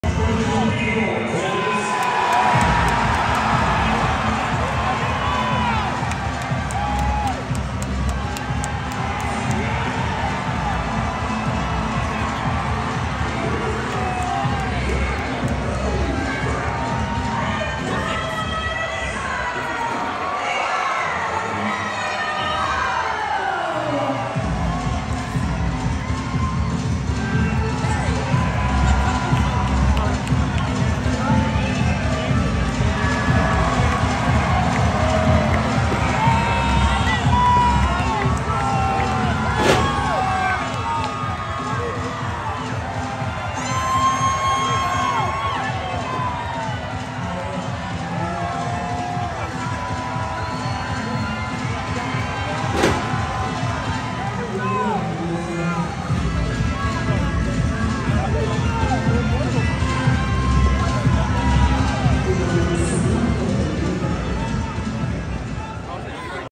Carlito entrance live - WWE Supershow 11/05/2023
Carlito makes his entrance for tag team action live at WWE Supershow